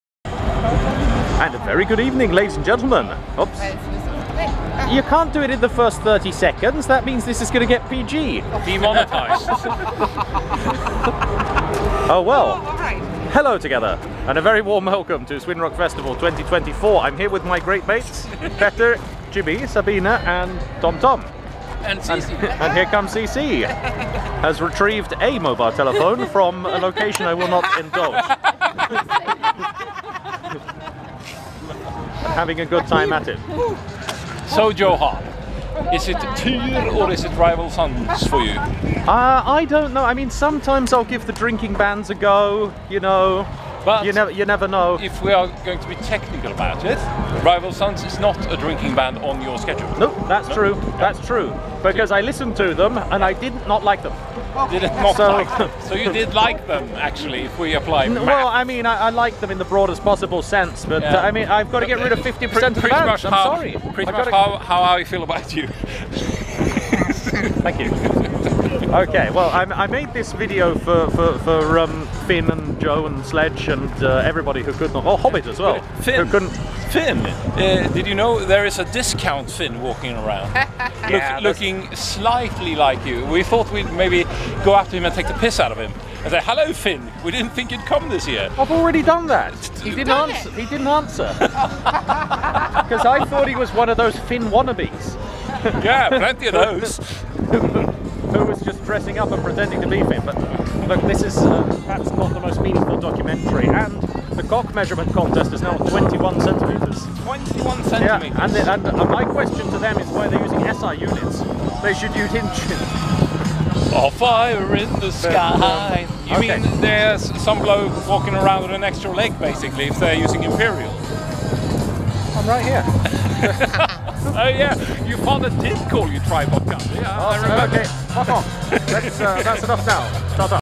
SRF2024 - Video Documentary Part 11 - One of those Fin wannabes
Wednesday at Sweden Rock Festival 2024, heading towards the festival area from Camp David for the second time with the gang...